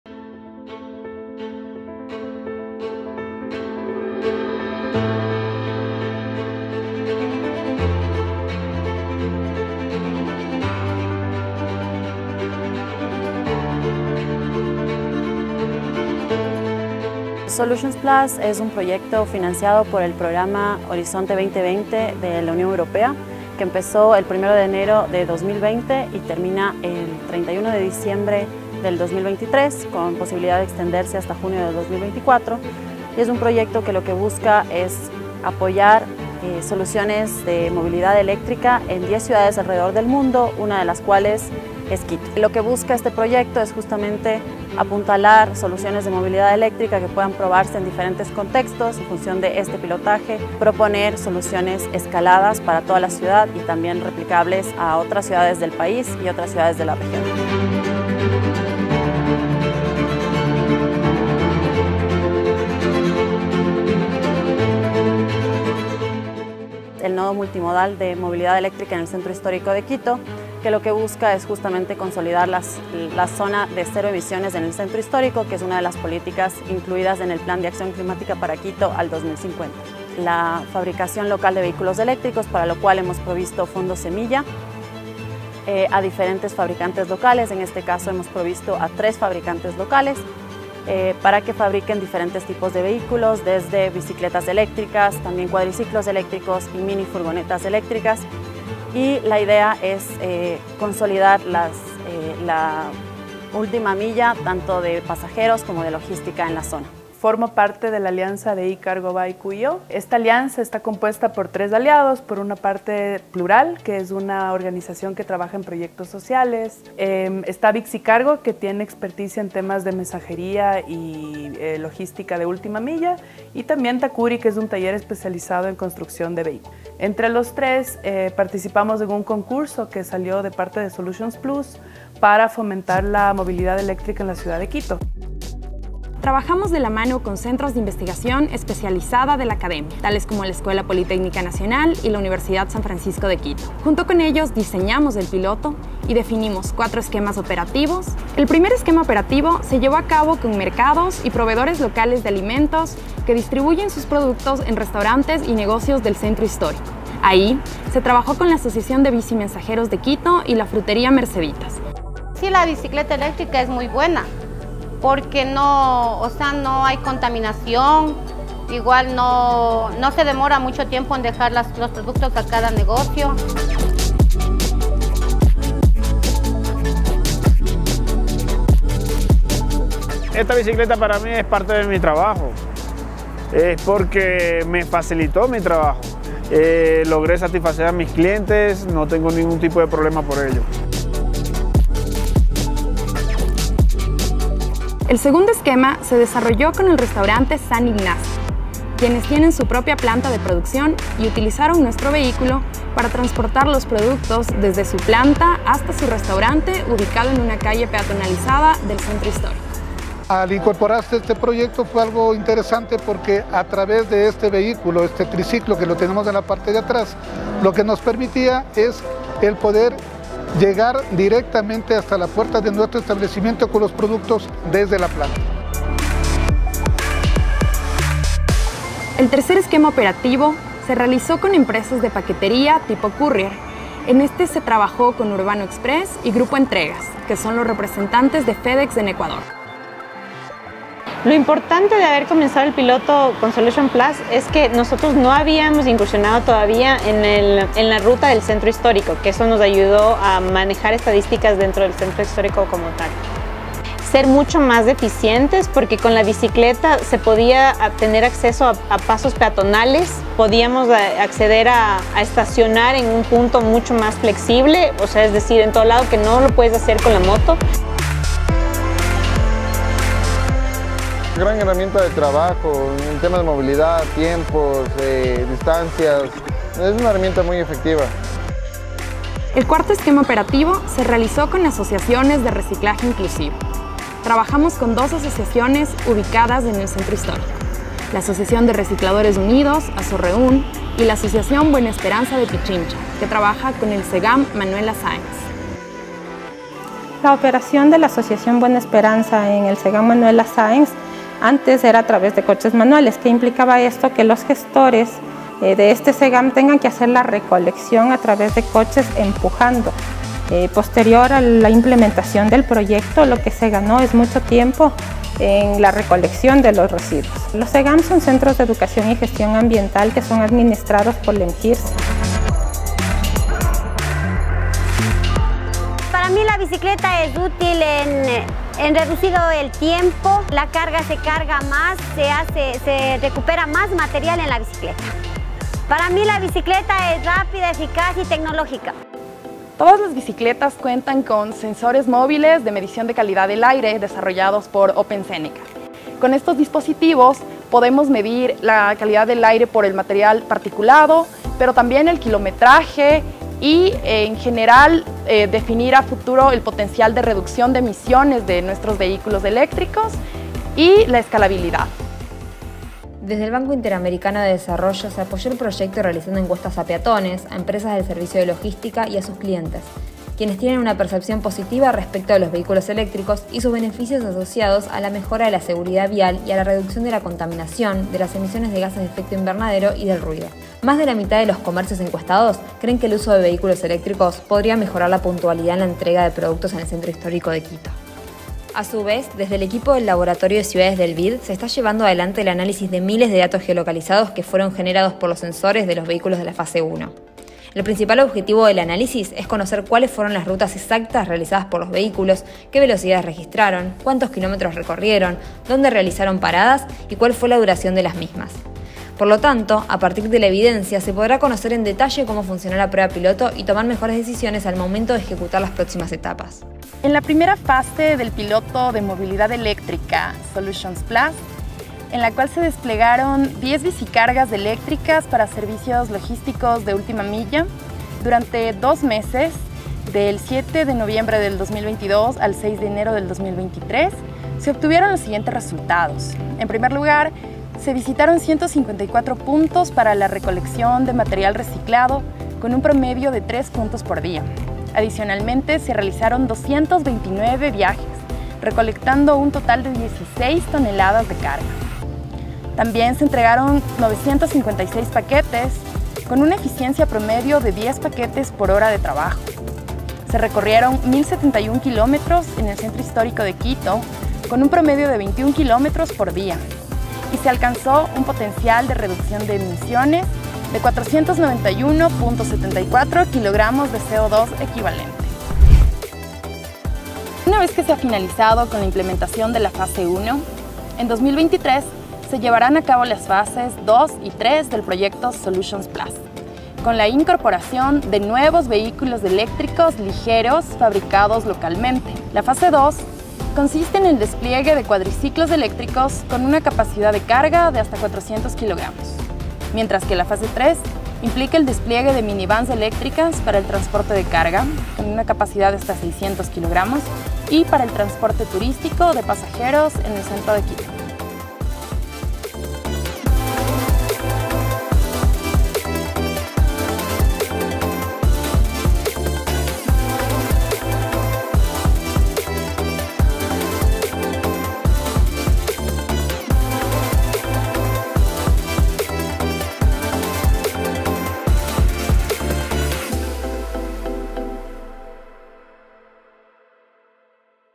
Movilidad eléctrica en la logística del Centro Histórico de Quito, Ecuador (proyecto SOLUTIONSplus)
El proyecto probó vehículos eléctricos livianos para actividades de reparto, paquetería y mensajería. El proyecto reunió a comercios del Centro Histórico de Quito, emprendimientos y empresas de logística y academia para estructurar 4 esquemas operativos para probar los vehículos eléctricos. Un proveedor local se encargó de la construcción de los vehículos eléctricos (bicicletas de carga de dos y tres ruedas).

Los vehículos contaban con sensores que medían la calidad del aire y registraban el trayecto con sistemas de información geográfica, lo que permitió reconstruir los recorridos y comportamiento de los conductores durante los dos meses de prueba. El Laboratorio de Ciudades de BID apoyó al proyecto SOLUTIONSplus analizando los datos generados por los sensores, así como realizando encuestas de percepción a peatones, usuarios del servicio de logística y empresas de logística.